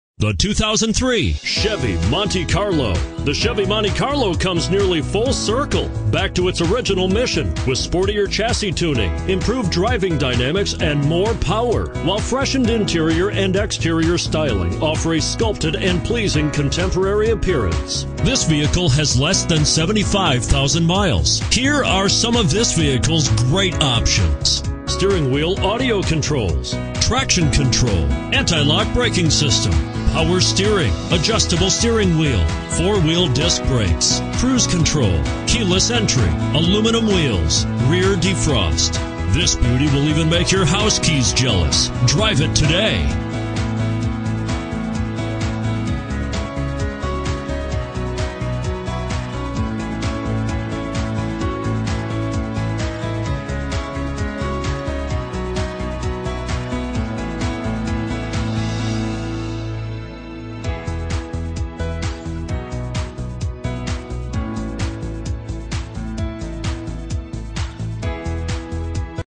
2003 CHEVROLET MONTE CARLO Old Bridge, NJ U88007
2003 CHEVROLET MONTE CARLO Old Bridge, NJ
Stock

Looking for the right car? Today could be your lucky day. With 74221 miles, this , 2003 Chevrolet Monte Carlo equipped with automatic transmission could be yours. Request more information and set up a test drive right away.